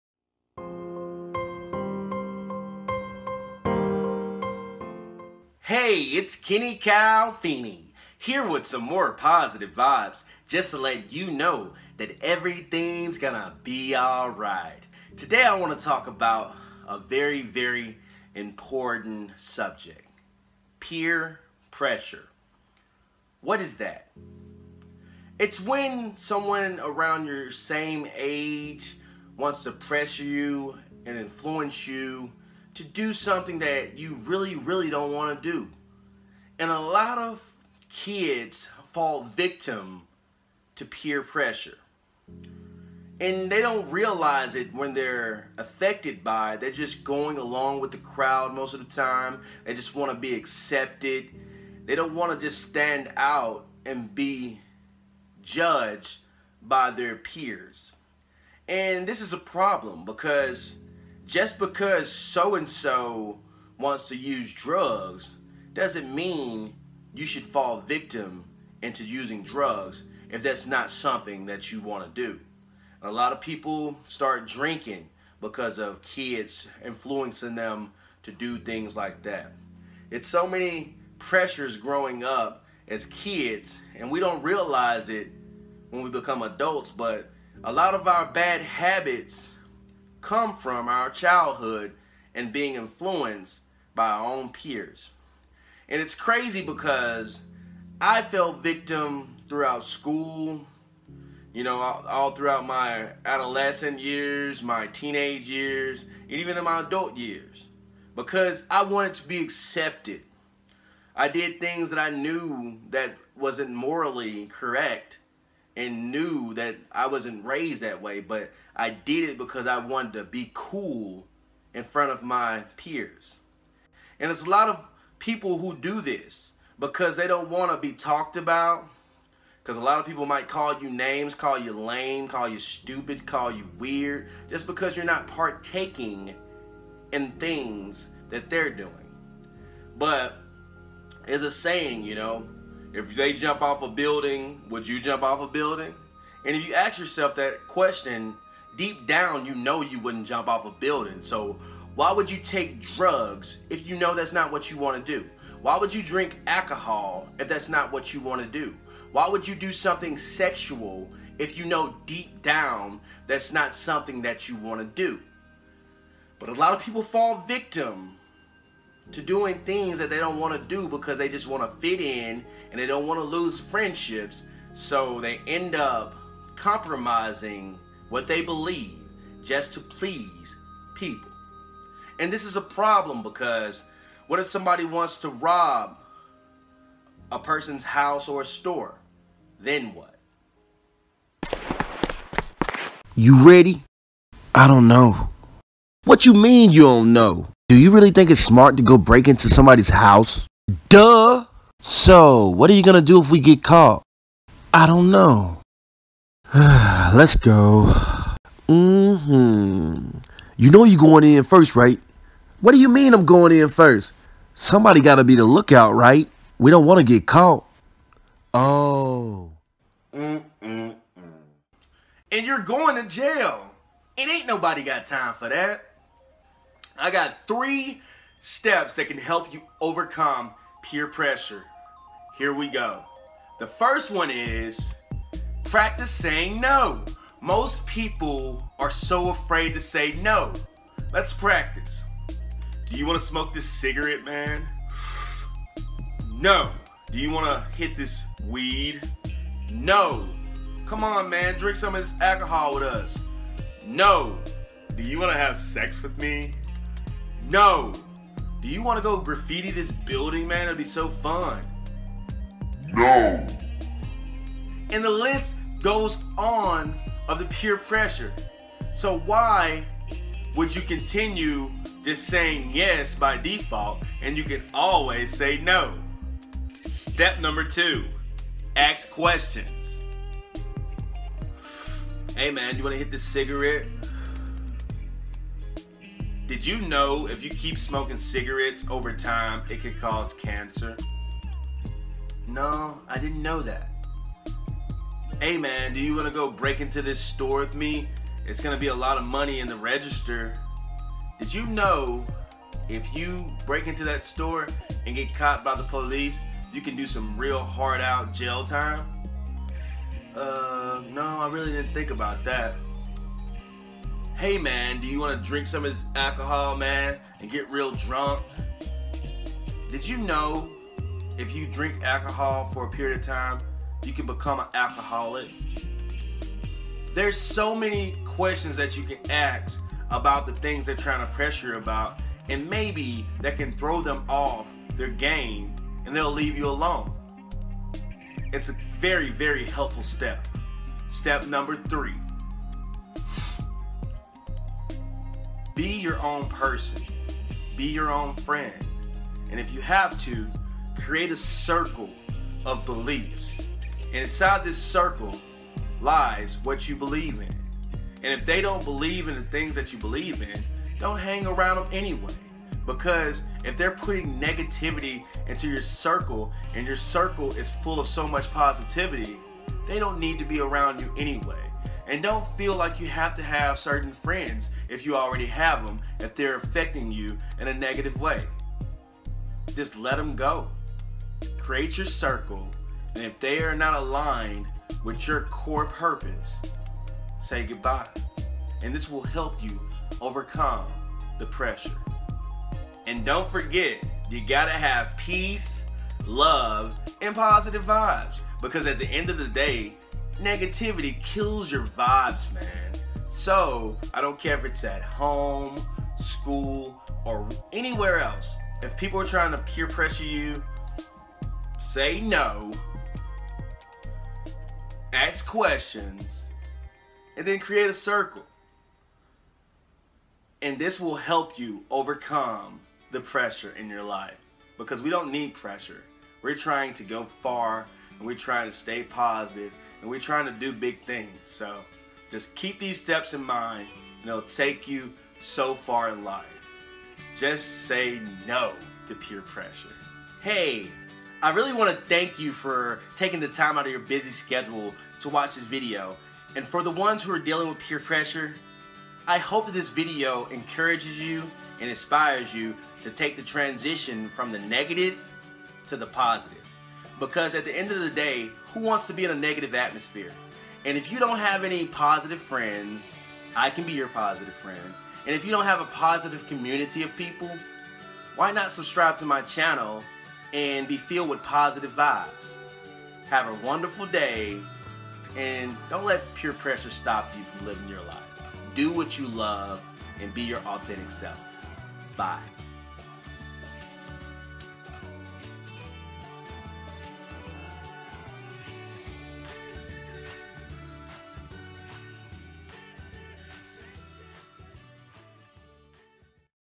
HOW TO STOP PEER PRESSURE (3 EASY TIPS)
Peer Pressure isn't a joke-it's very real for teens and adults alike. Teens are even more vulnerable to peer pressure, and it can get to the point where some teens are driven to the point of trying drugs and alcohol or other unhealthy habits because of it. It's important that we spread positive tips to avoid peer pressure!